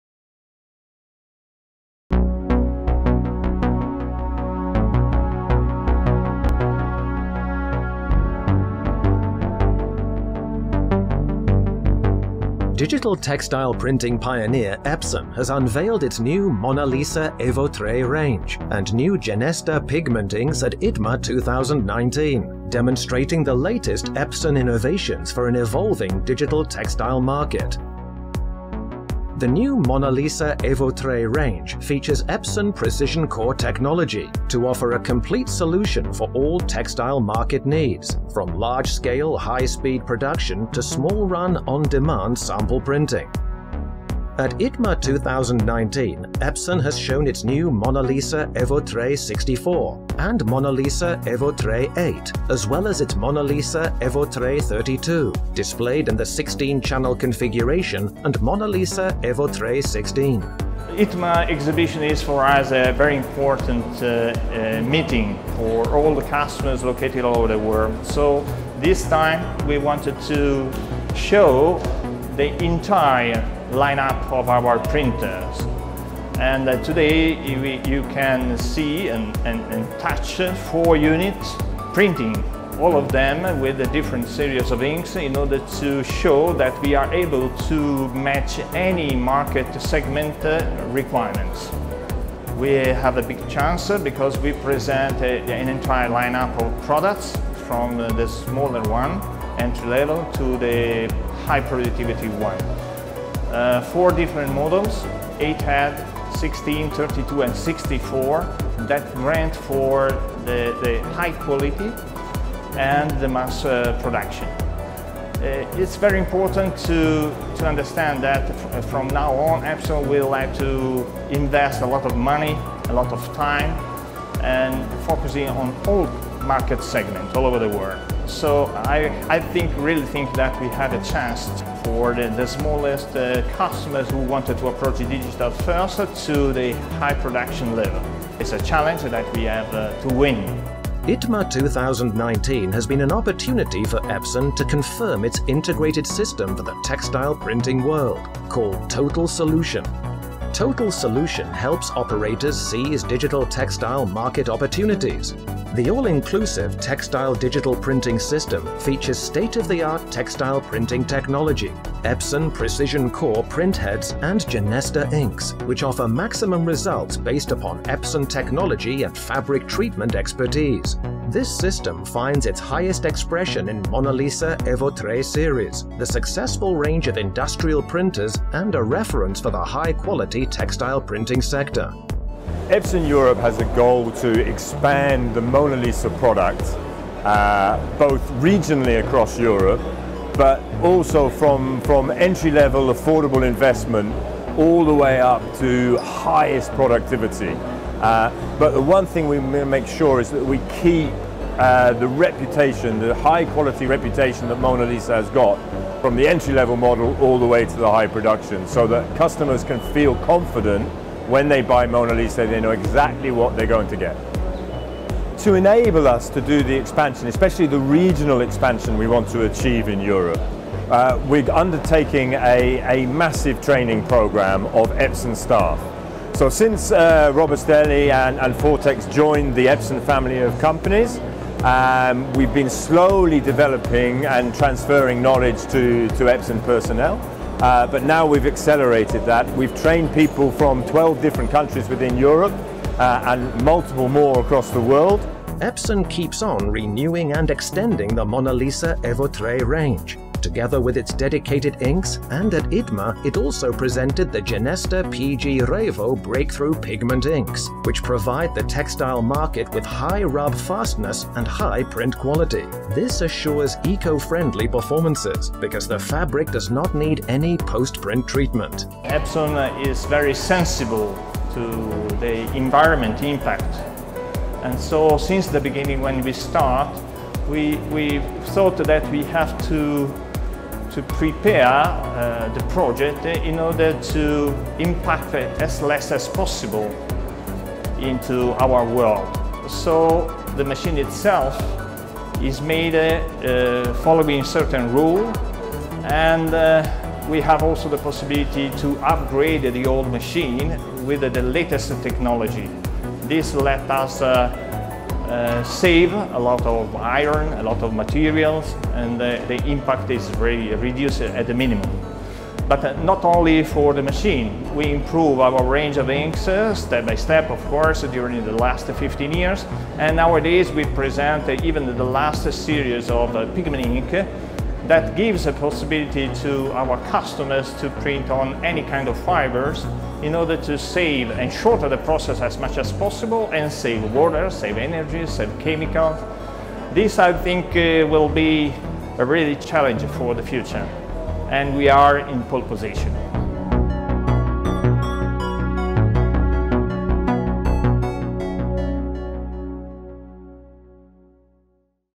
Epson at ITMA 2019. A revolution in digital textile printing
Digital textile printing pioneer, Epson has unveiled its new Monna Lisa Evo Tre range and new Genesta pigment inks at ITMA 2019, demonstrating the latest Epson innovations for an evolving digital textile market.
The new Monna Lisa Evo Tre range features Epson PrecisionCore Technology to offer a complete solution for all textile market needs, from large scale, high speed production to small run, on demand sample printing.  At ITMA 2019, Epson has shown its new Monna Lisa Evo Tre 64 and Monna Lisa Evo Tre 8, as well as its Monna Lisa Evo Tre 32 (displayed in the 16 channel configuration) and Monna Lisa Evo Tre 16.